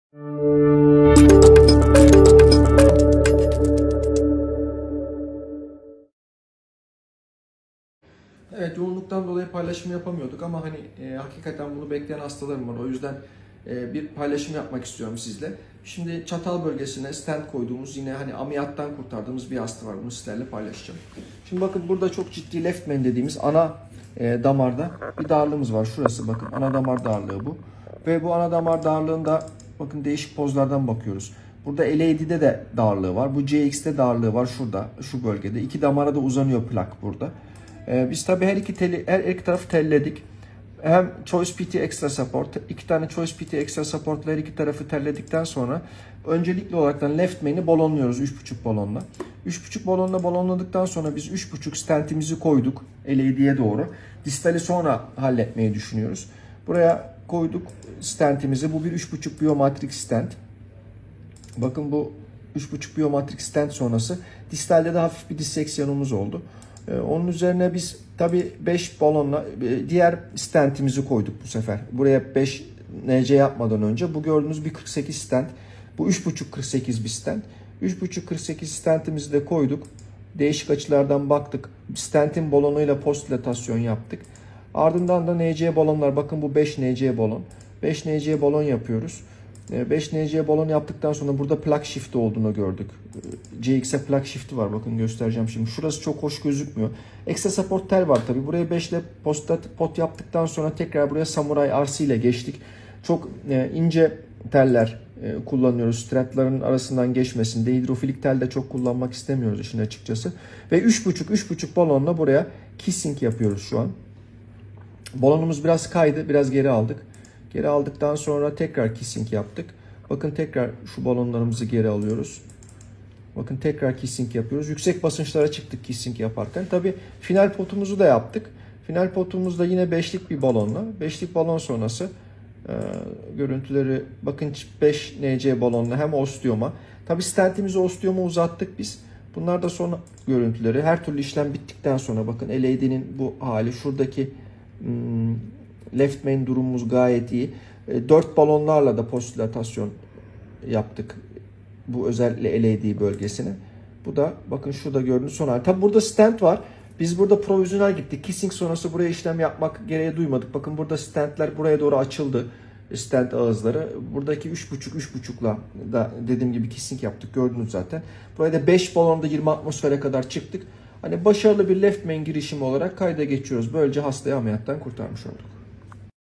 Hastamızın Sağ Başparmağından Girerek Tıkalı Damarlarını Stentle Açtık - Prof. Dr. Şevket Görgülü
Hastamızın sağ başparmağından girerek tıkalı damarlarını stentle açtık. Böylece ameliyata gerek kalmadı (distal radial, LMCA intervention).

Prof. Dr. Şevket GÖRGÜLÜ

Kliniğimizin Adresi:
Fenerbahçe, Operatör Cemil Topuzlu Caddesi Sedef Apartmanı No:31 K:1 D:2, 34726 Kadıköy/İstanbul